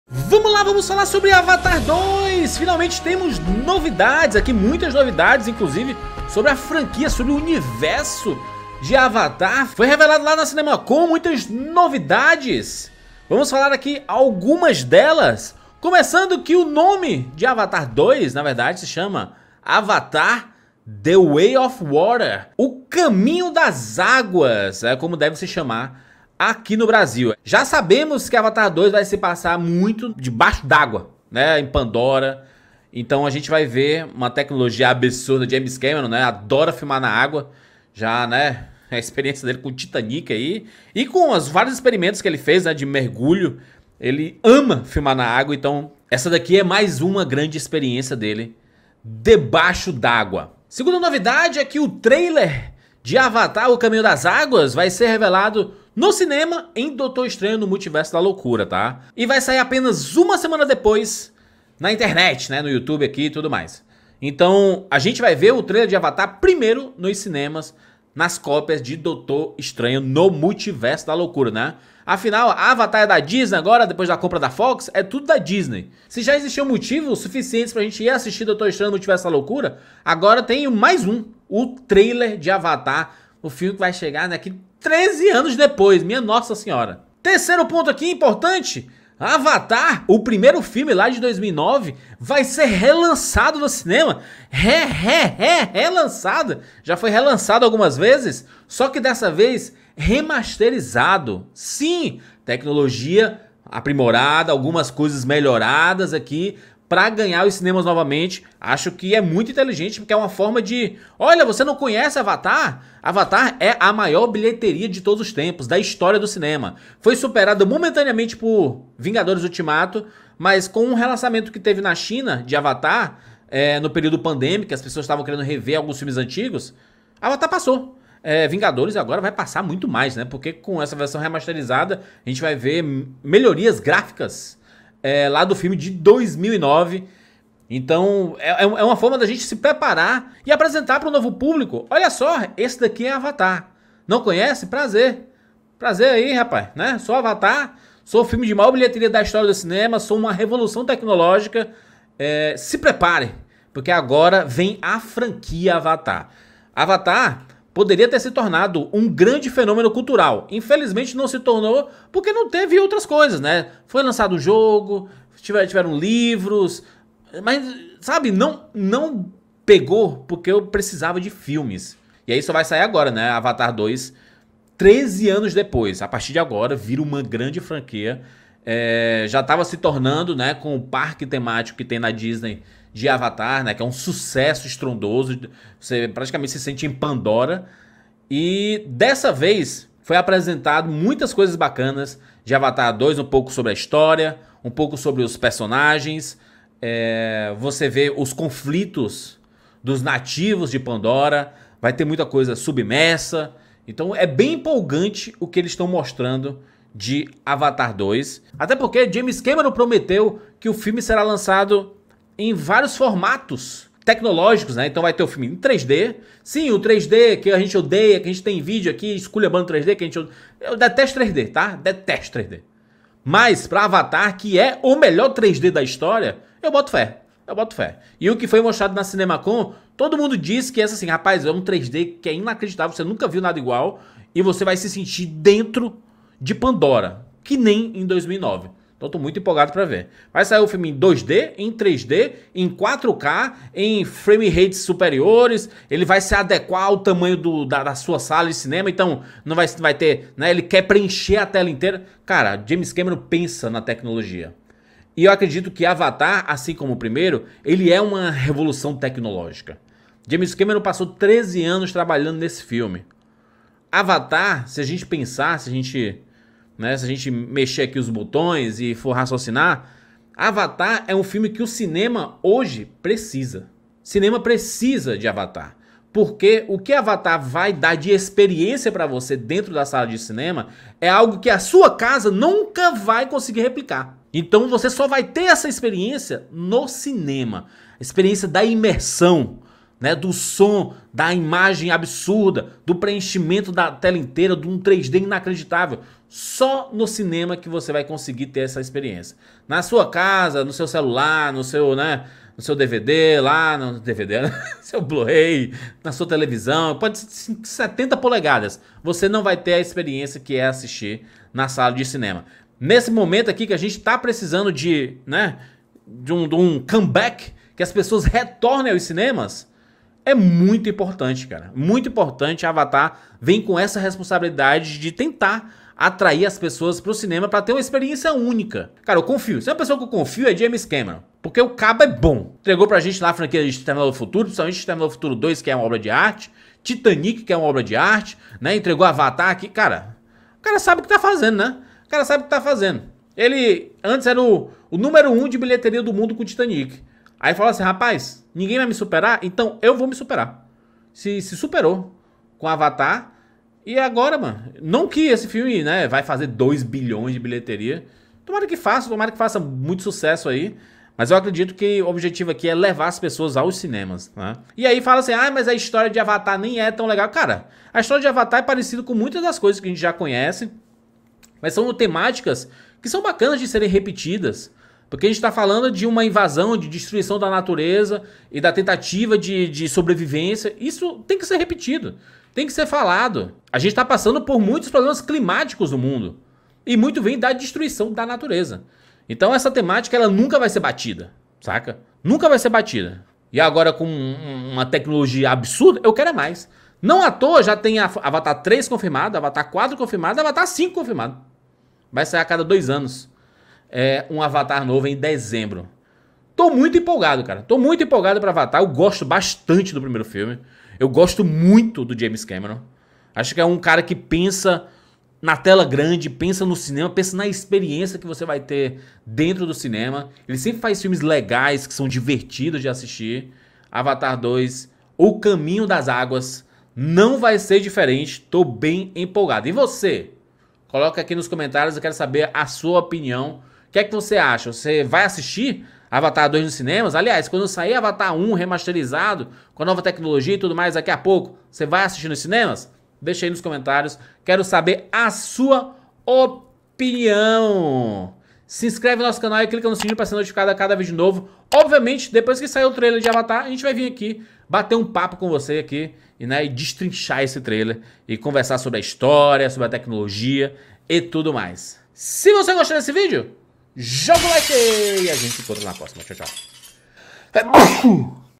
AVATAR 2 é o filme que o cinema precisa!
A tão aguardada sequência de “Avatar” ganhou título oficial: “Avatar: O Caminho das Águas”. O primeiro trailer chega com "Doutor Estranho 2", O primeiro Avatar vai ser relançado remasterizado. MUITAS NEWS! "Avatar 2" é um filme que o cinema precisa. Por que?

Siga o RAPADURA por aí!